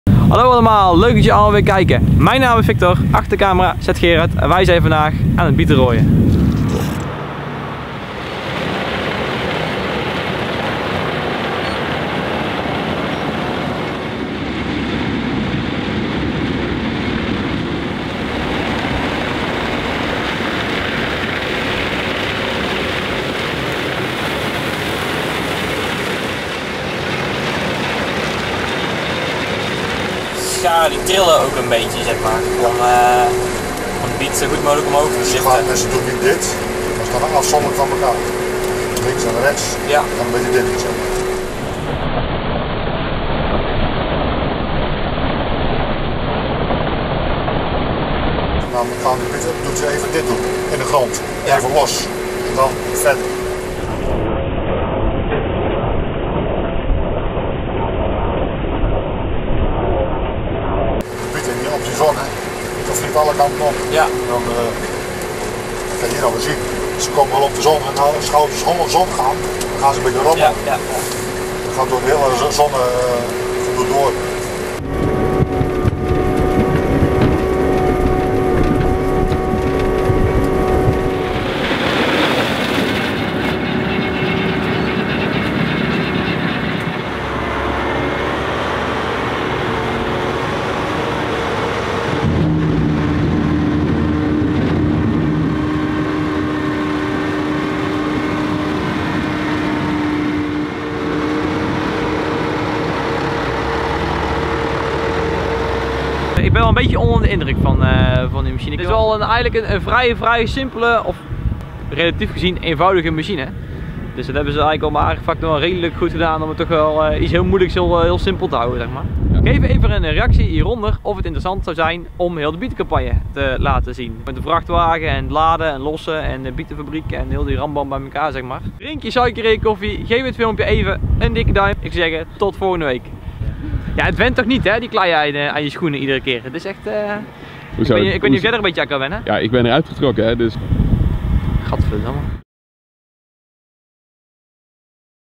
Bieten rooien met Cintegro - AgriTraderTV #12
Bietenrooiers zijn machtige machines waarbij de eenvoud steeds belangrijker is geworden.

Wil jij graag meer filmpjes van de bietencampagne zien? Laat dat even in een reactie weten!

Deze video is gemaakt bij Cintegro in Ens - bedankt mannen!